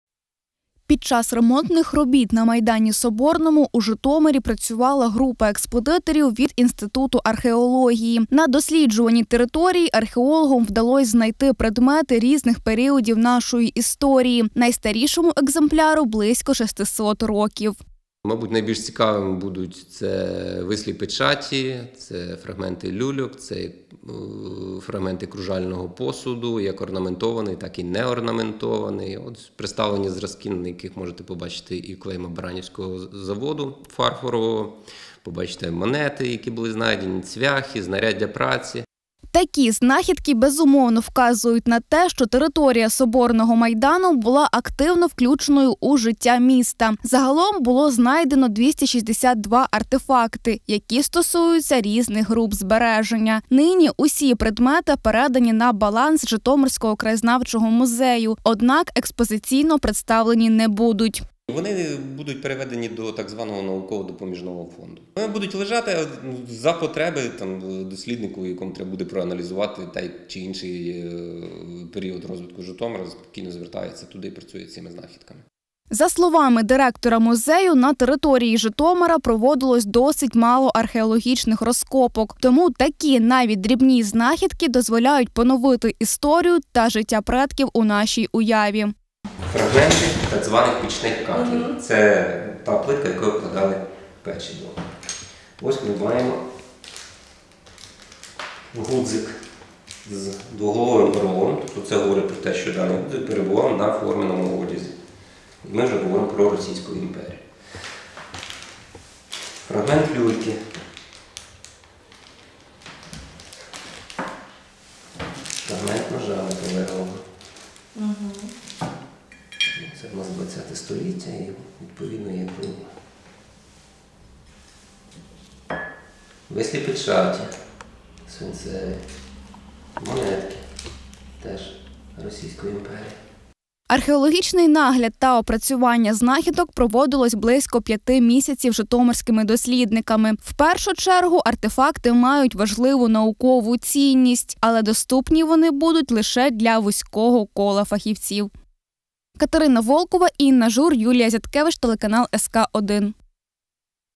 Під час реконструкції скверу на майдані Соборному знайшли близько 300-х археологічних знахідок
Під час реконструкції скверу на майдані Соборному у Житомирі знайшли близько 300-х археологічних знахідок, деяким з них більше 500 років. Серед них фрагменти ножів, посуду, люльок та пічні кахлі. Зараз знахідки минулого передали до краєзнавчого музею